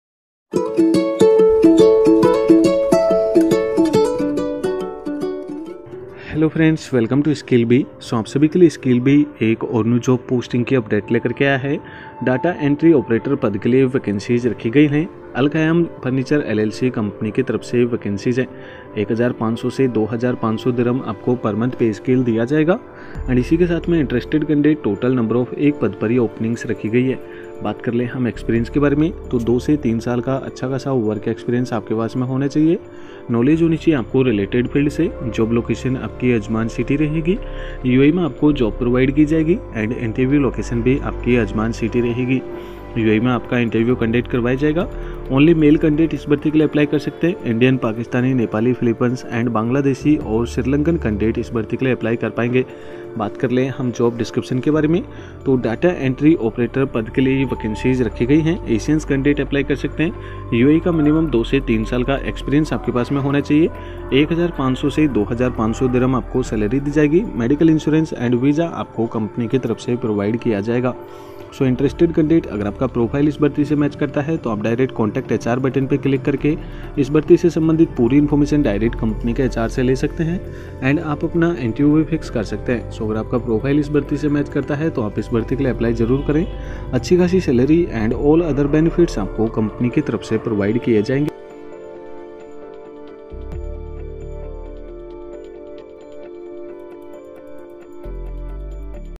JOB IN UAE/ Role: Data Entry Operator/ Company: Al Khayam Furniture LLC/Salary: 1500-2500
13...Check out this job opening on the Skillbee app that is available for you:

Role: Data Entry Operator

Company: Al khayam furniture llc

Openings: 1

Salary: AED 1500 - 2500

Experience: 2 - 3 years

Job Location: Ajman - United Arab Emirates

Interview Location: Ajman - United Arab Emirates